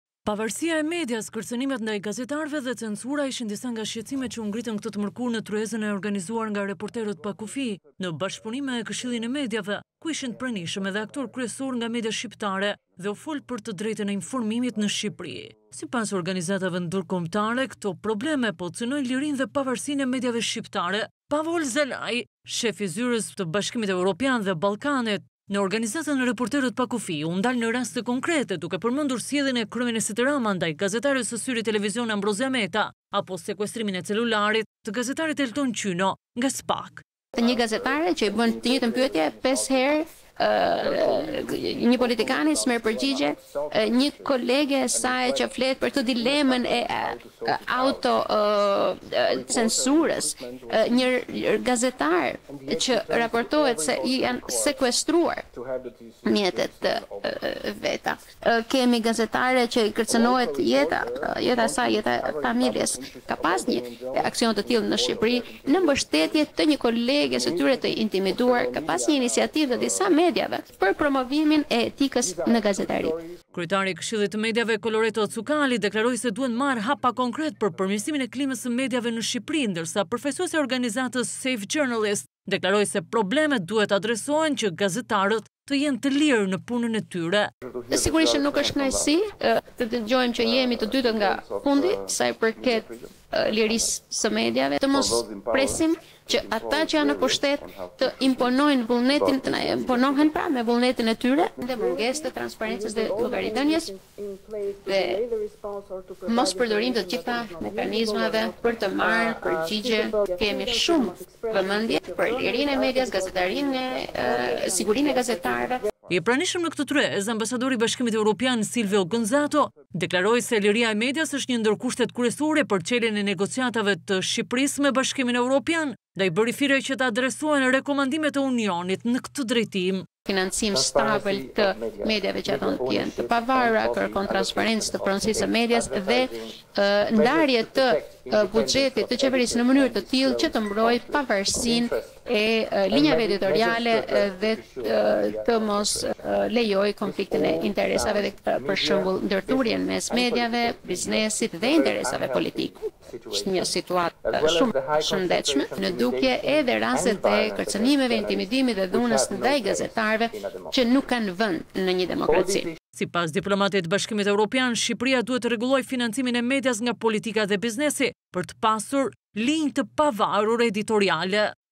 Organizatat e medias shqetësim për fjalën e lirë, Cukali:Jemi të dytët nga fundi për lirinë e medias
Organizatat ndërkombëtare  dhe Këshilli shqiptar i mediave ngrene shqetësImin për  kërcënimet ndaj gazetarëve, pavarsinë dhe censurën mediatike, që sipas tyre po cënojnë fjalën e lire. Shefi i zyrës së BE në Organizatën 'Reporterët pa Kufij' solli raste konkrete të kërcënimeve dhe intimidimit të gazetarëve,  duke u ndalur tek Ambrozia Meta dhe Elton Qyno. Ndërkohë ambasadori i BE, Silvio Gonzata bëri thirrje që të adresohen këto probleme, duke theksuar se liria e medias është kusht kryesor për celjen e negociatave me Unionin